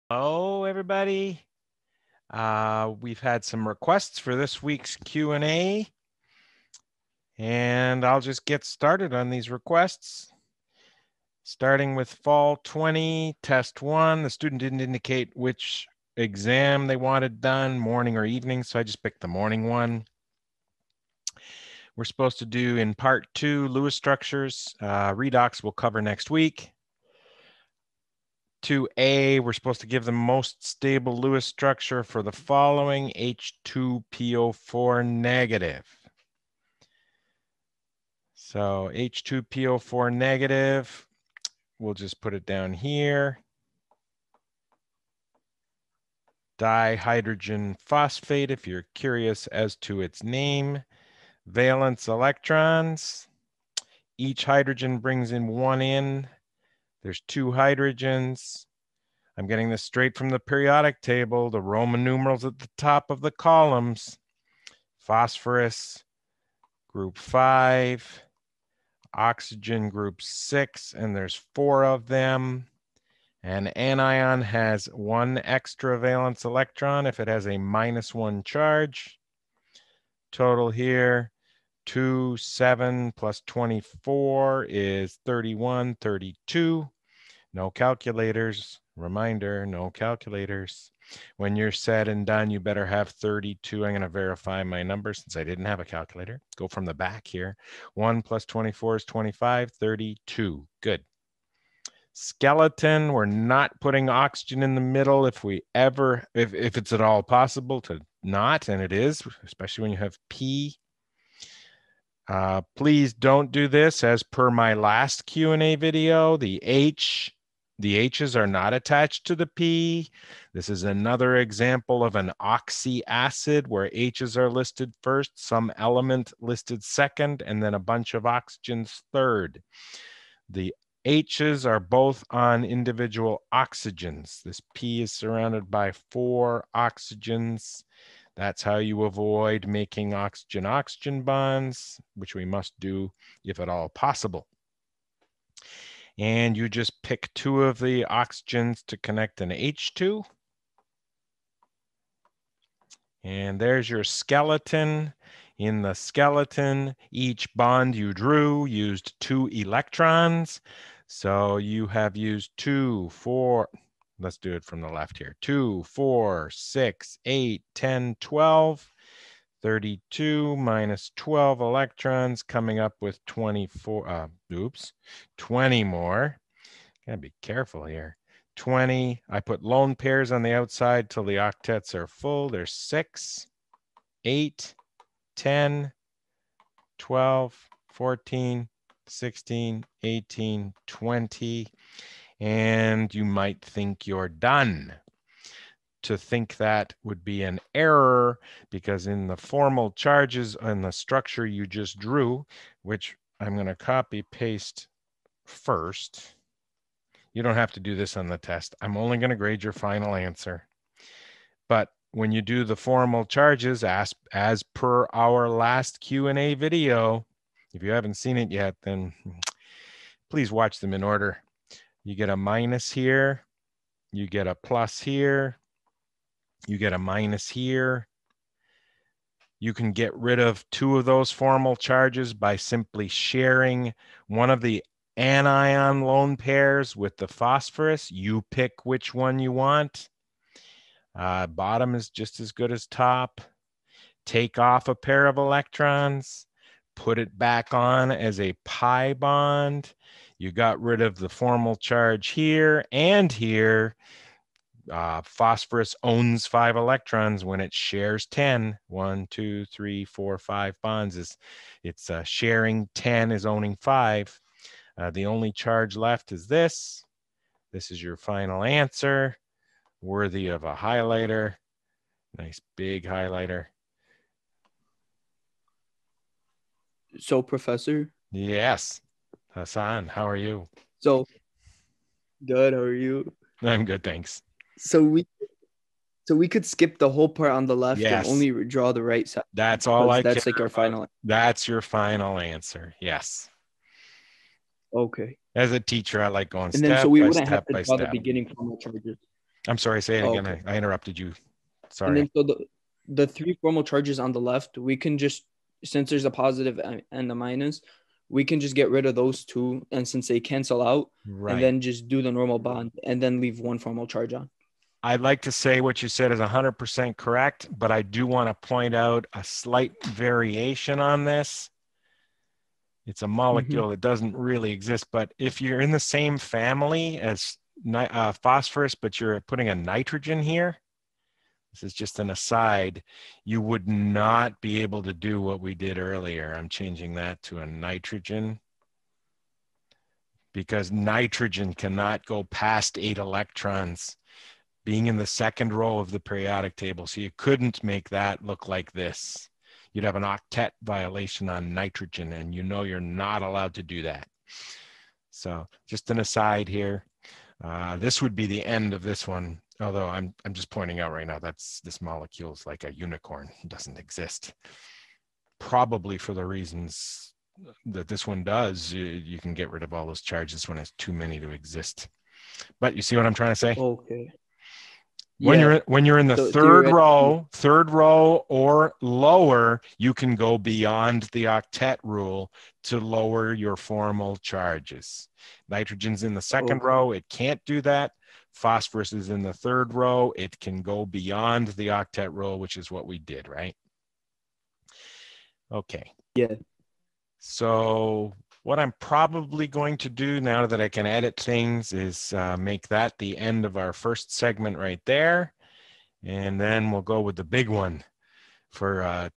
Draw the Lewis structure for dihydrogen phosphate
Draw the Lewis structure for dihydrogen phosphate, H2PO4-
Accompanying document: